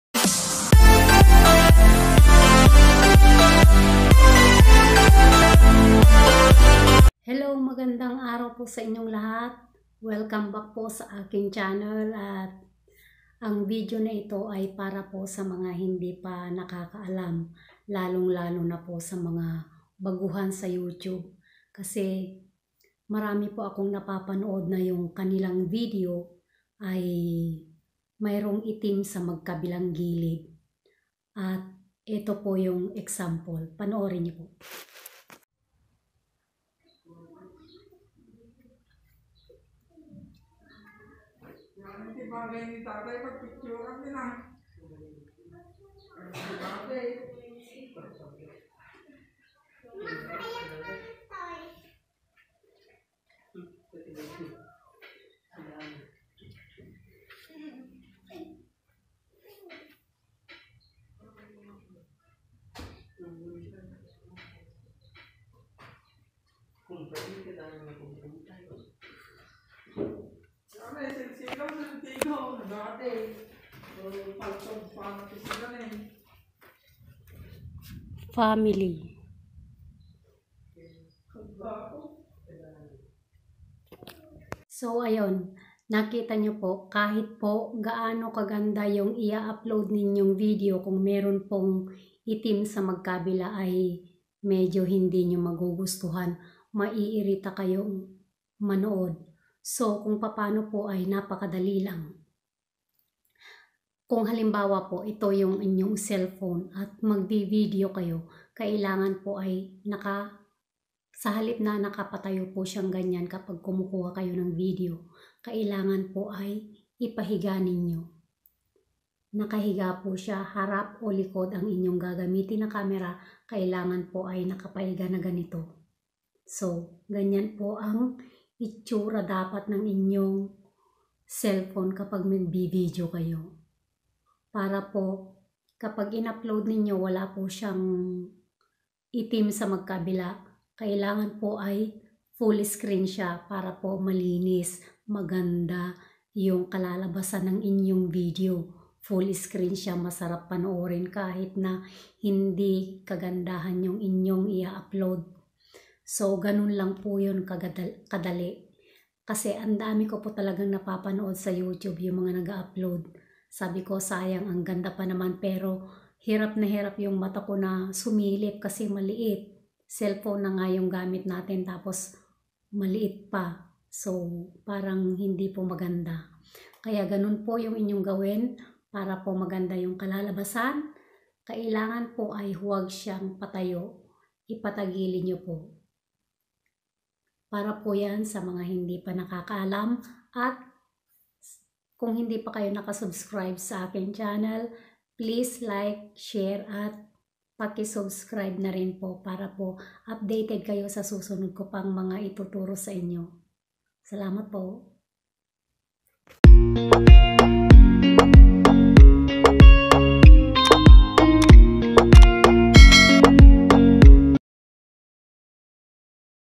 MALING PAG VIDEO NG MGA BAGUHANG BLOGGER
maraming bagung blogger ngayon na di alam ang tamang pag video kaya makitid at may itim sa magkabilang gilid ang kanilang video